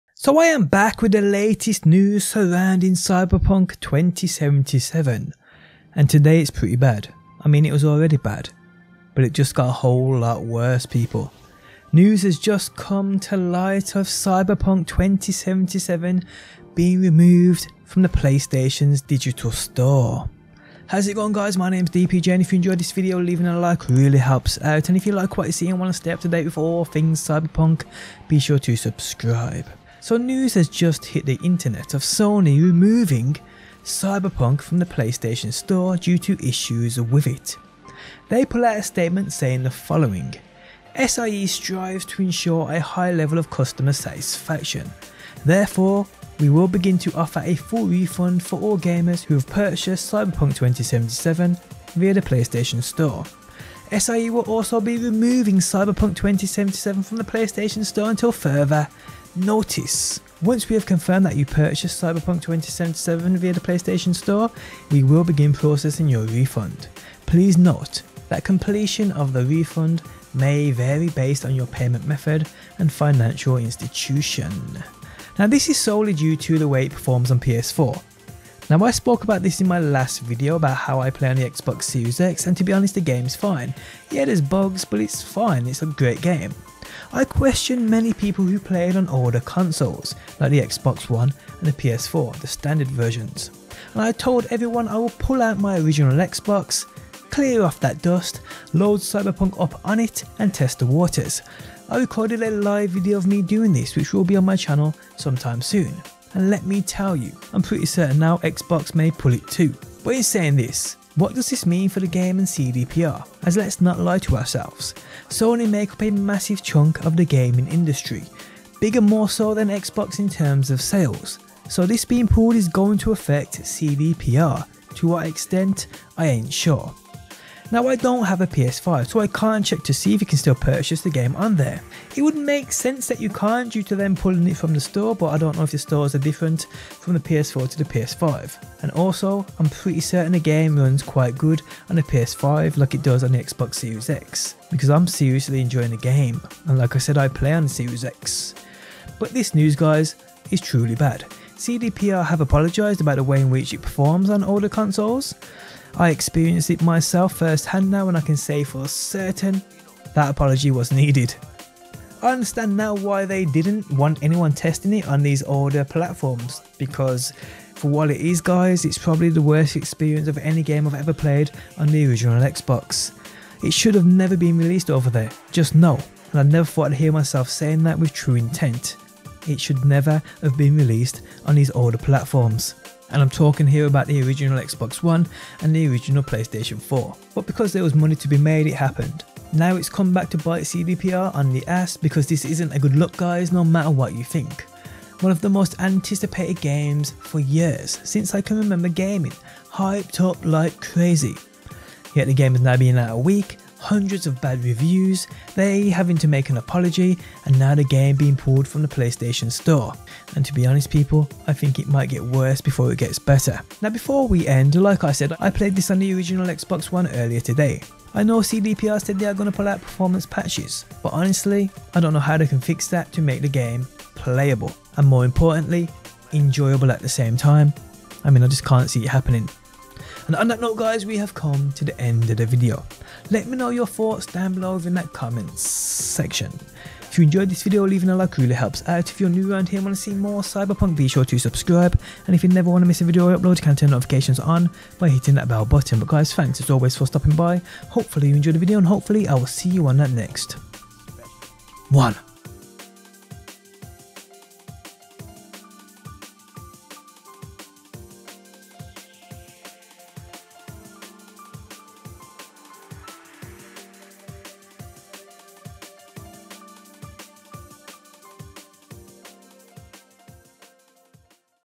SONY REMOVED CYBERPUNK 2077 FROM THE PS STORE! - Cyberpunk 2077 News
Sony have removed Cyberpunk 2077 from the playstation store